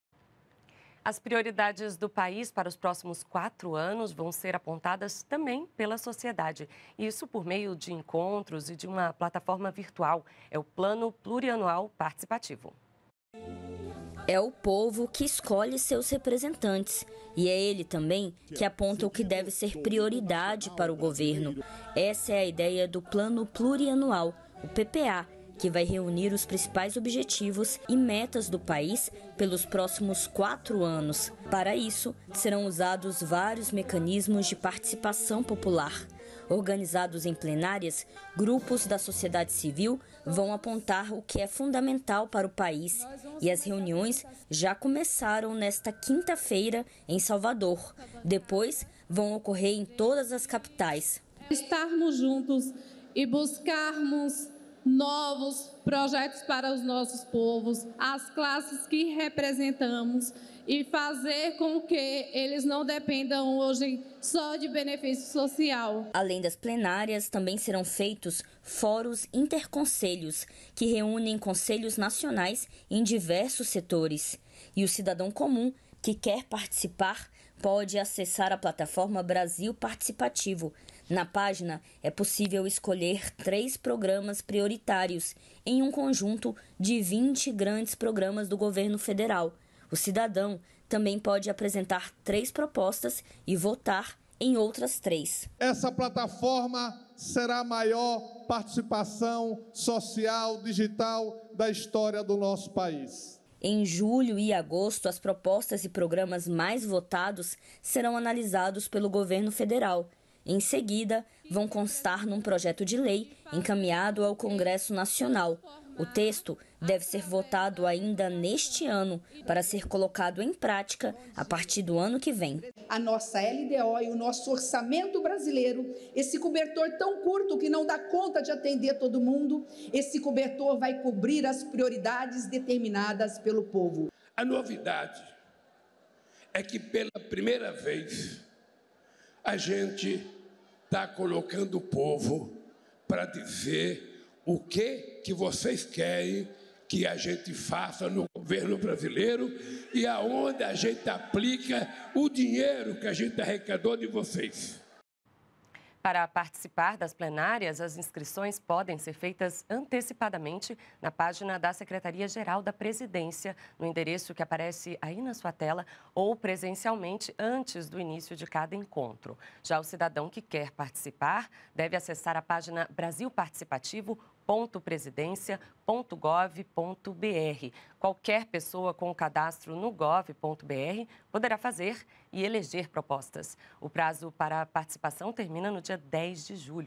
PPA: PRIORIDADES SERÃO INDICADAS PELA SOCIEDADE
12.05.23 - As prioridades do país para os próximos quatro anos vão ser indicadas pela sociedade. É o Plano Plurianual Participativo.